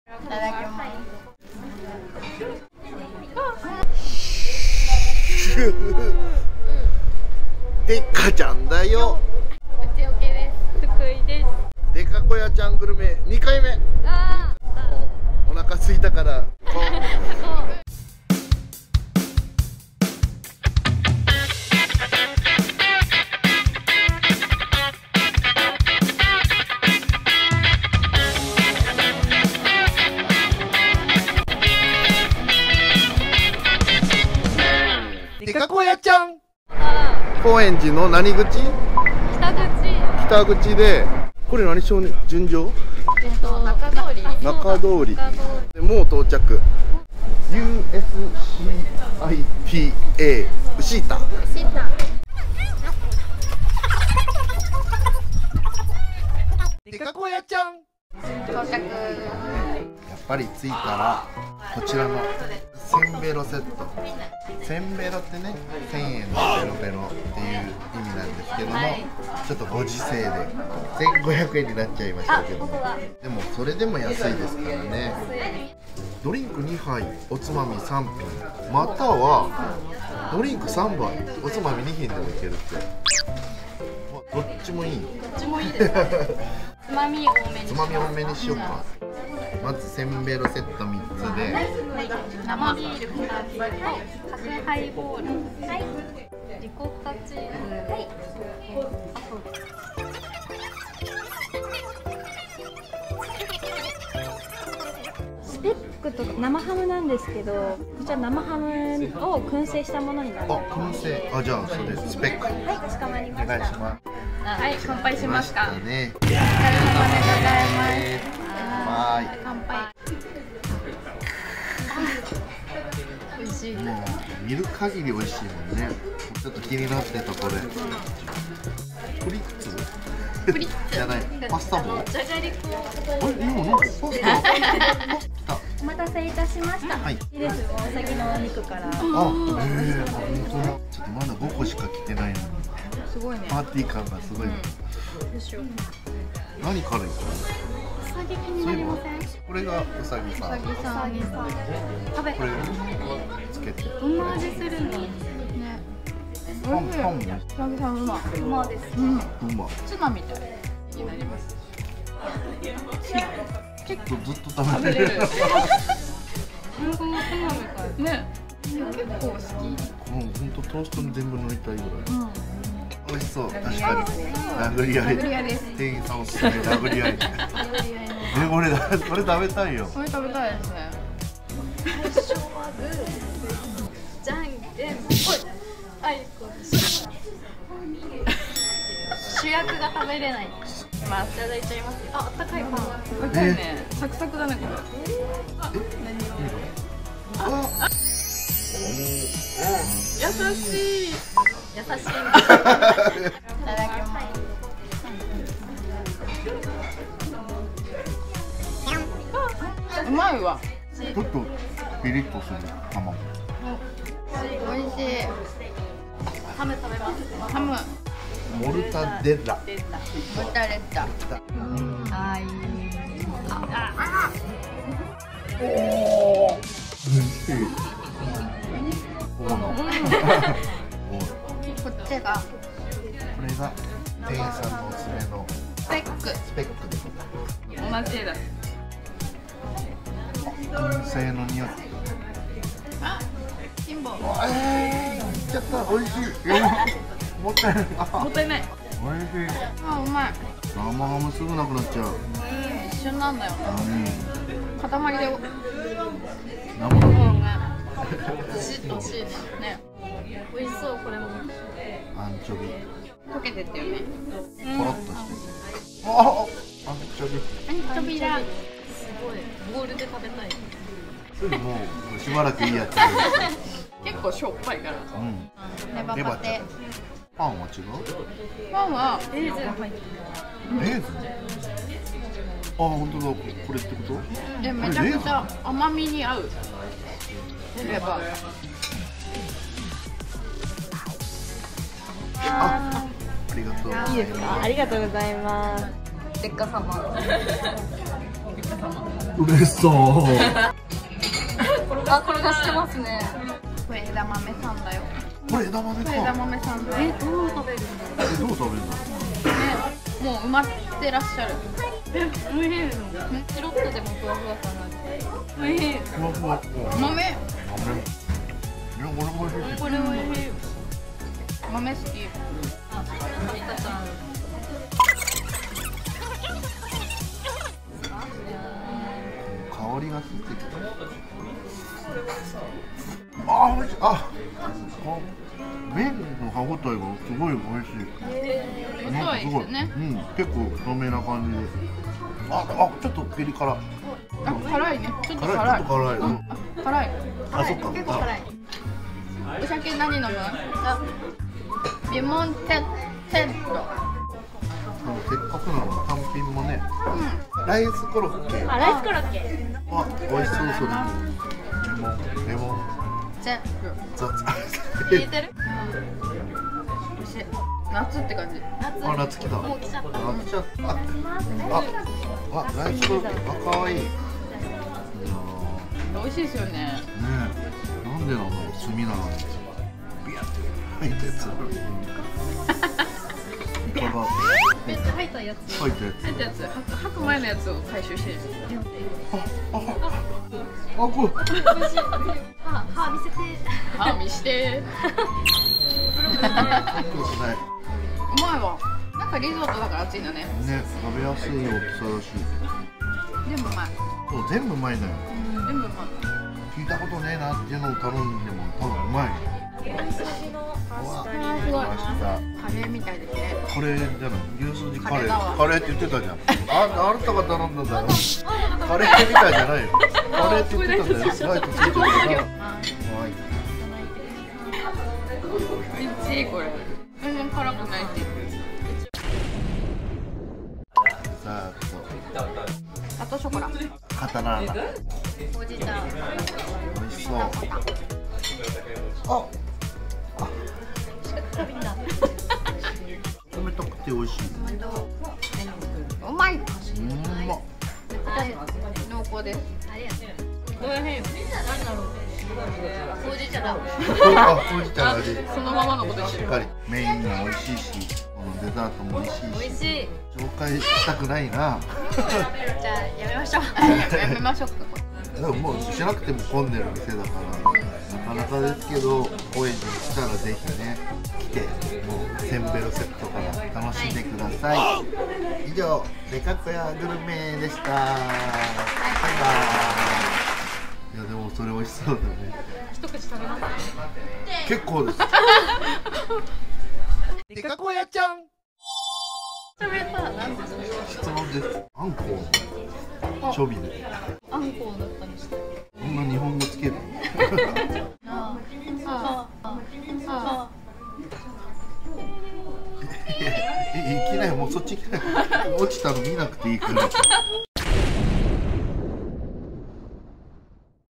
【高円寺】至高のせんべろウシータを堪能！（アンディアーモ アッラ ウシータ/Andiamo all' Uscita ）
デカコヤチャンの番外編！
スタジオ・コヤーマの店員さんと高円寺のグルメを食べ尽くす。

デカコヤグルメの第2回目！
アンディアーモ アッラ ウシータ/Andiamo all' Uscita (高円寺/イタリアン)
高円寺駅徒歩90秒、絶品パスタが自慢のコスパ最高イタリアン！

浅草開化楼特製【カラヒグ麵】使用の絶品パスタが人気です

月～土11:00~22:30
(最終入店21:00/フードL.O.21:30/ドリンクL.O.22:00)
日曜日のみ17:00閉店
月に1回程度で不定休あり
最新情報はTwitterをご覧ください。